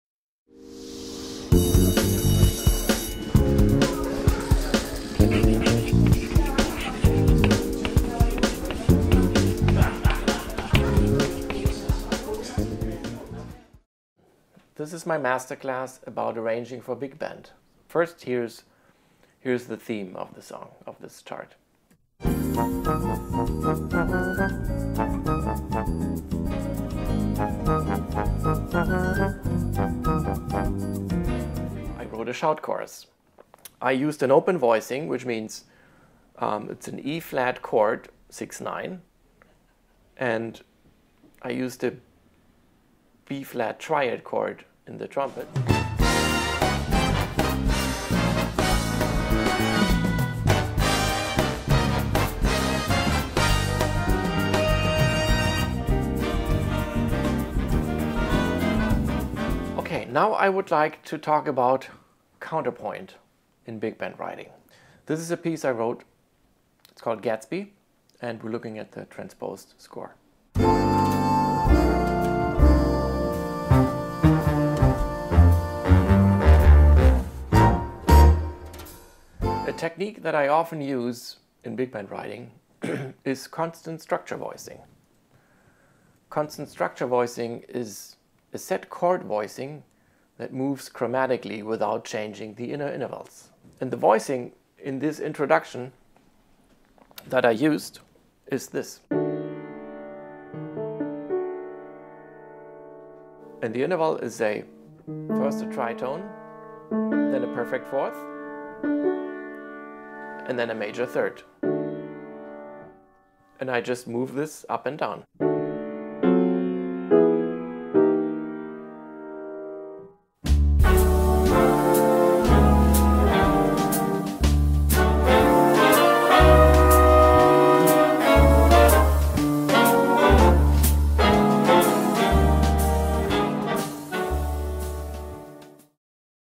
Chris Walden - Arranging for Big Band
Description: In this "Arranging for Big Band" masterclass, renowned composer and arranger Chris Walden shows you some tricks of the trade. Chris discusses and demonstrates many crucial concepts including score format, articulation, notation practices, voicing options, counterpoint and more. If you want to get some arranging tips from one of the industry's elite, this masterclass is for you. NOTE: There is a PDF bundled with the download version of this video (not the $11.99 rental version) which includes supporting notation.

Topics Covered: Arranging, Big Band, Score Format, Staff Order, Articulation, Rhythm Notation, Drum Notation, Voicing Options, Interval Limits, Shout Chorus, Counterpoint, Tips and Tricks, Etc.